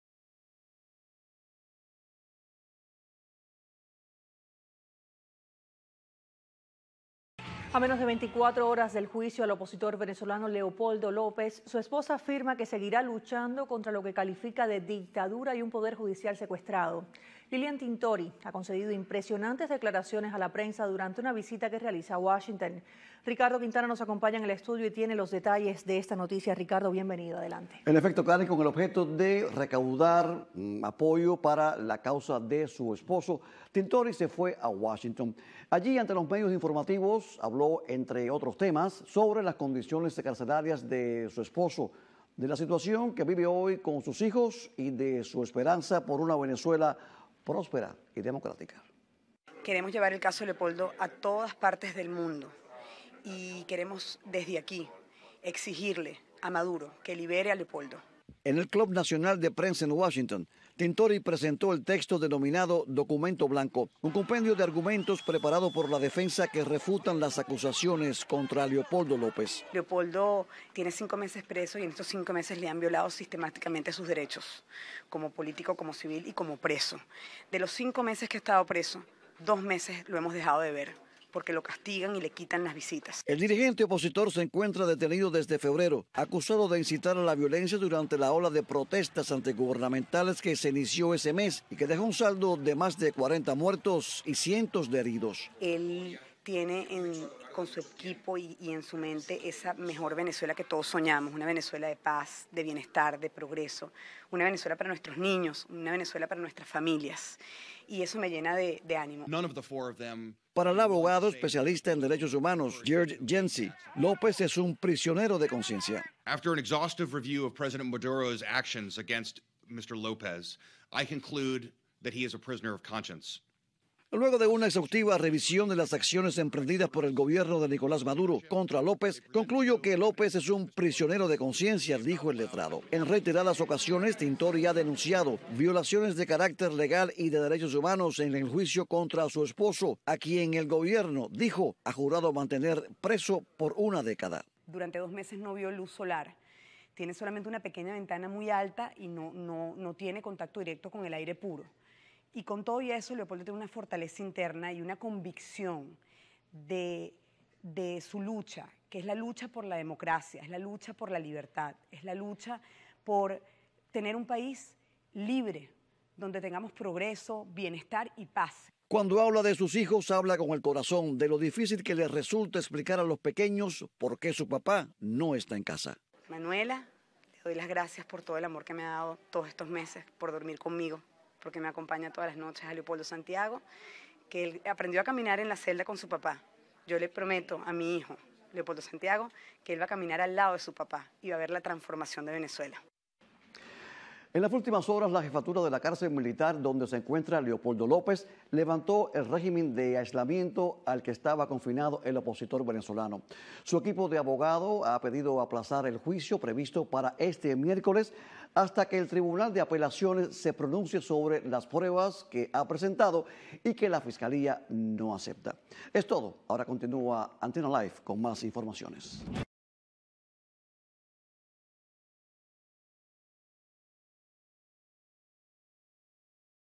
Esposa de opositor venezolano Leopoldo López recaba apoyo en Washington
En Washington, Lilian Tintori, esposa del opositor venezolano, Leopoldo López, asegura que seguirá luchando contra lo que califica de dictadura y un poder judicial secuestrado. Ricardo Quintana tiene sus declaraciones.